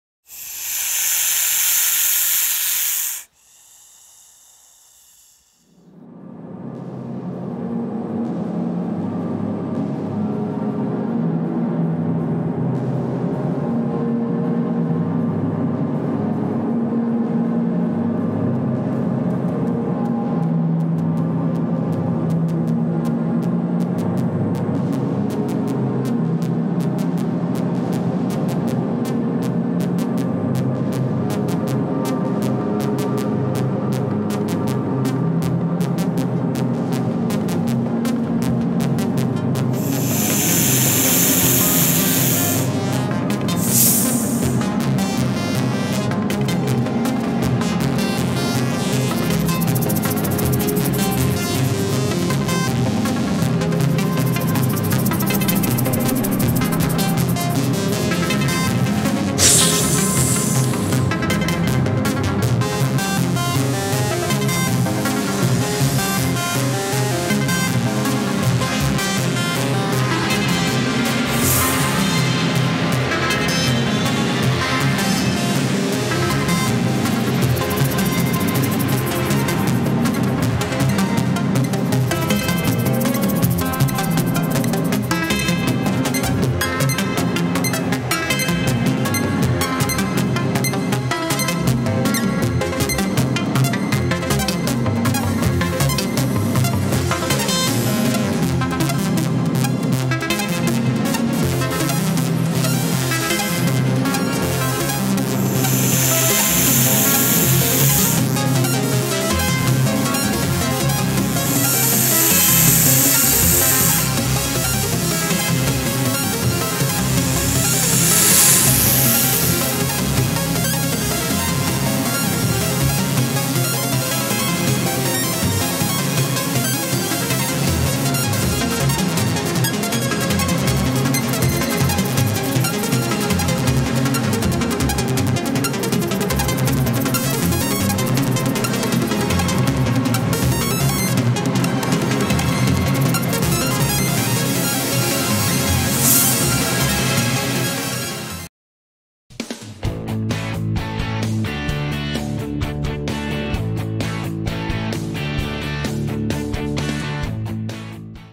Big Python In Real Life HD Video 2
In this video you are going to see Big Pyton Snake in Real Life.

Comment Your Favourite Part. And don’t forget Likes & share the video.
VFX & Edit by Tarun Tanwar
Staring by Sagar Bhadana

 Music and Sounds from Production Crate.
 Some sounds are recorded separately.
This video is shoot on Iphone 14 Pro
 Software Used Blender 3.1, Davinci Resolve and After Effects